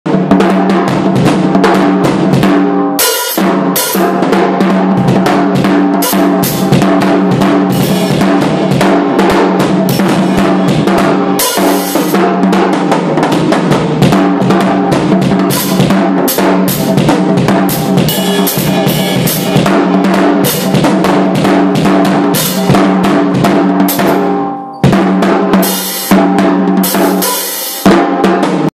KYLE BROWN ON DRUMS
Just sheddn a lil n the studio... INDIANA DRUMMERS...we out here too frfr.